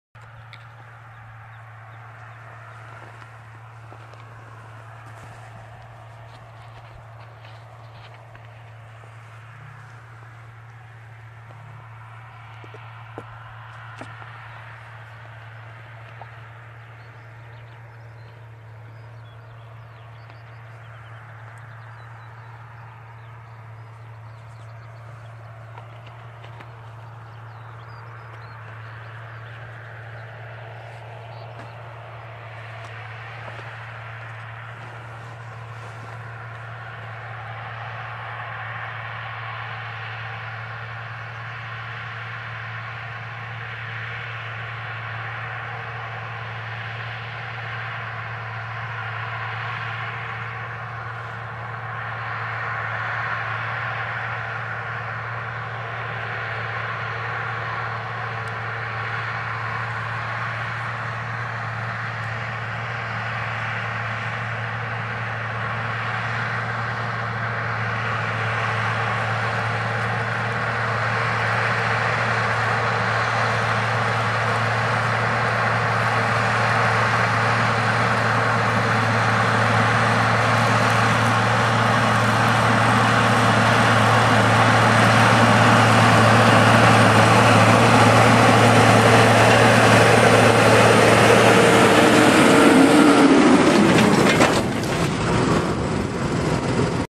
Полосовая обработка почвы (Strip-till) культиватором Orthman
Технология обработки почвы "Стрип-тилл" предусматривает полосовое рыхление на 25 см в ширину и до 30-35 см в глубину, две трети поля остается в нетронутых междурядьях. В данной технологии соединились преимущества отвальной пахоты (прогрев и просушка почвы) с защитой грунта, благодаря тому, что рыхлятся только полосы для внесения семян. Кроме того, при этой технологии  можно внести удобрения на нужном уровне для каждой культуры.